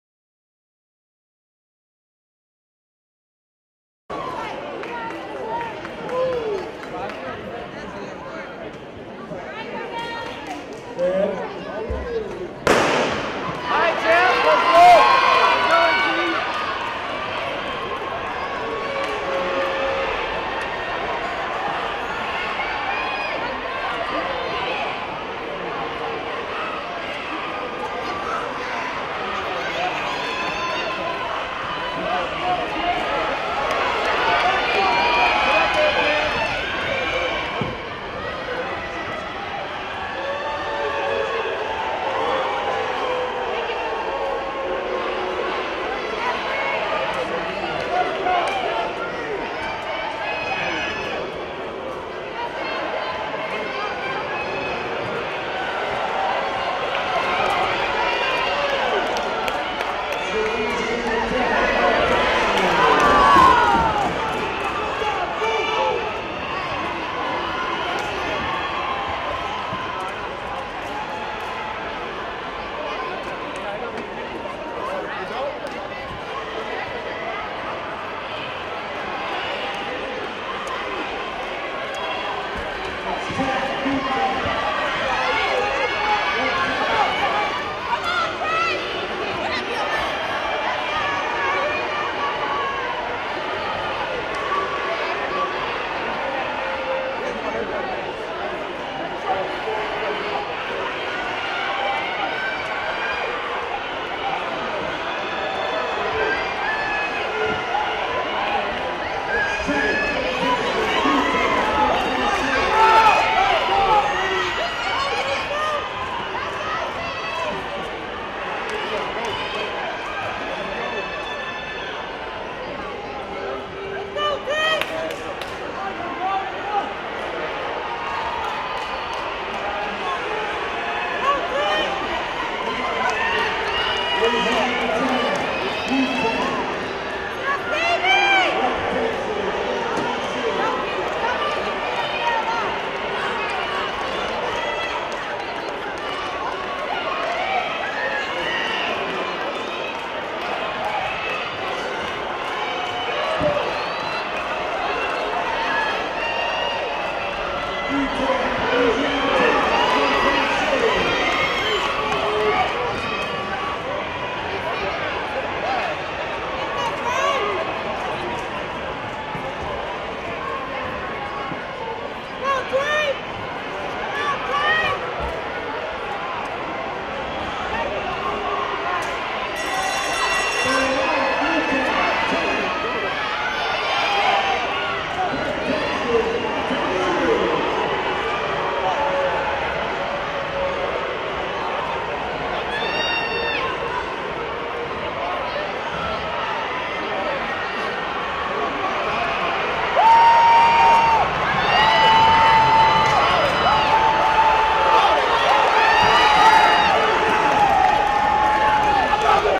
Men's 4x400m Relay win Gold
Watch LA Tech's men's 4x400m relay win gold in dramatic fashion